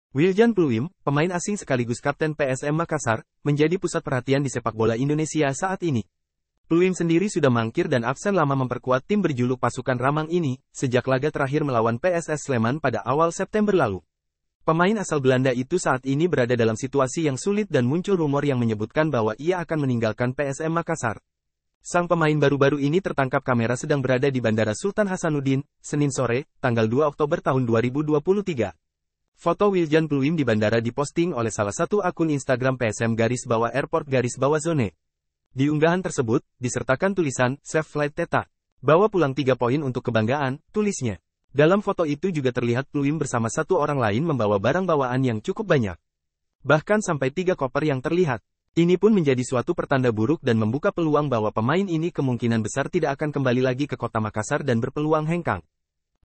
Wiljan Pluim Hengkang dari PSM Makassar?
Wiljan Pluim, pemain asing sekaligus kapten PSM Makassar, menjadi pusat perhatian di sepak bola Indonesia saat ini.

Pluim sendiri sudah mangkir dan absen lama memperkuat tim berjuluk Pasukan Ramang ini, sejak laga terakhir melawan PSS Sleman pada awal September lalu.

Pemain asal Belanda itu saat ini berada dalam situasi yang sulit dan muncul rumor yang menyebutkan bahwa ia akan meninggalkan PSM Makassar.

Sang pemain baru-baru ini tertangkap kamera sedang berada di Bandara Sultan Hassanuddin, Senin sore (2/10/2023) lalu.

Foto Wiljan Pluim di bandara diposting oleh salah satu akun instagram @psm_airport_zone.

Di unggahan tersebut, disertakan tulisan "Safe Flight Tetta!!! Bawa pulang 3 poin untuk kebanggaan!!!," tulisnya.

Dalam foto itu juga terlihat Pluim bersama satu orang lain membawa barang bawaan yang cukup banyak. Bahkan sampai tiga koper yang terlihat.

Ini pun menjadi suatu pertanda buruk dan membuka peluang bahwa pemain ini kemungkinan besar tidak akan kembali lagi ke Kota Makassar dan berpeluang hengkang.